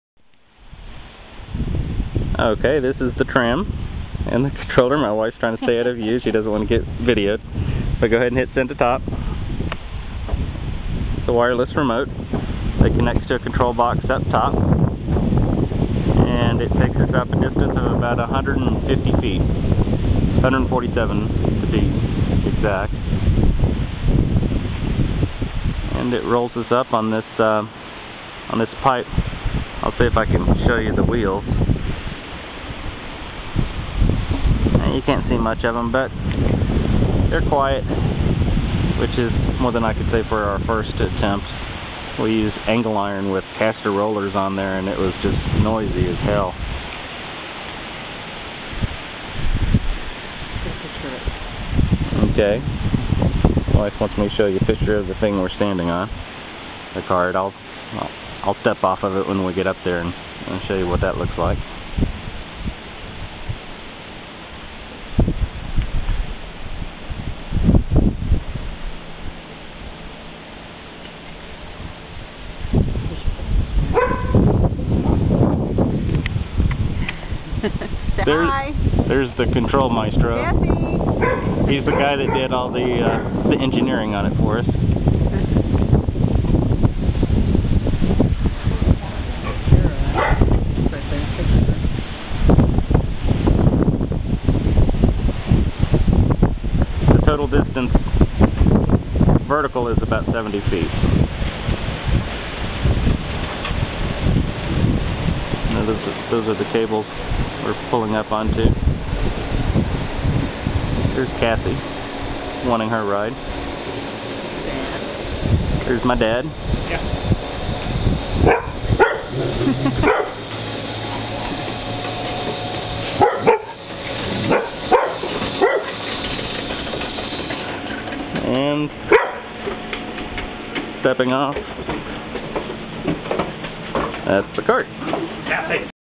Tram tour
Here's a tour of the hill-tram that Dad and I built.  My bro-in-law Ken was a huge help as well.  My apologies to Holly for catching her on camera without makeup.  Also, much thanks to Jud Rice for the welding expertise.